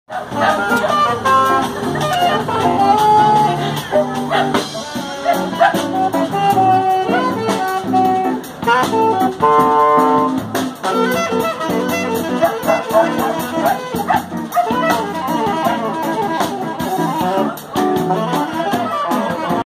Cachorro vira-lata se revolta com banda que toca na Avenida Paulista
Os latidos chamaram mais atenção que a própria música